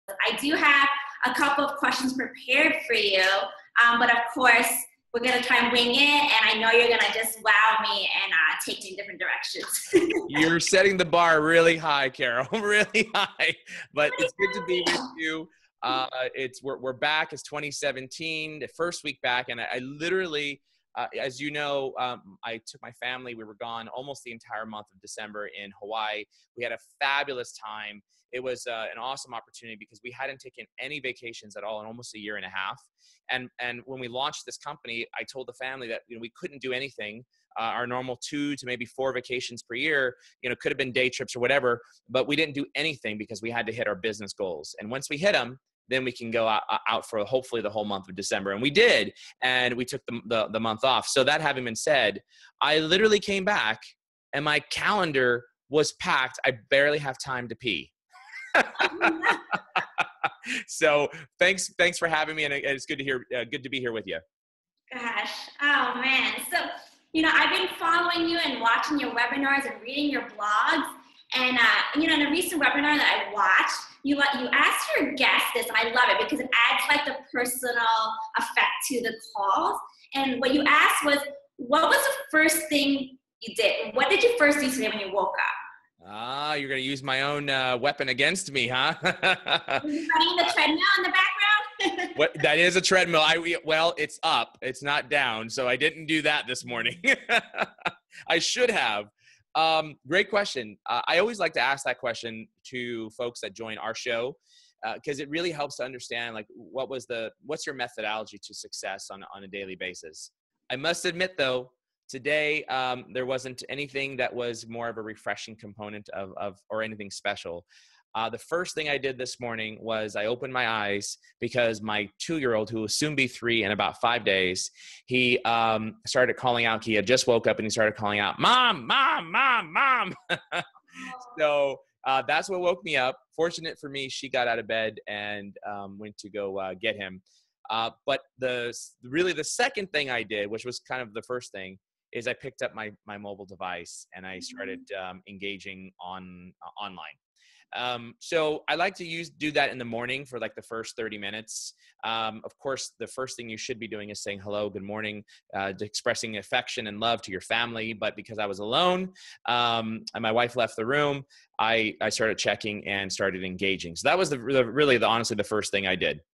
Getting Personal With Mario Martinez Jr.
Gain insight into how this social selling expert starts his day and stays laser-focused on reaching business goals!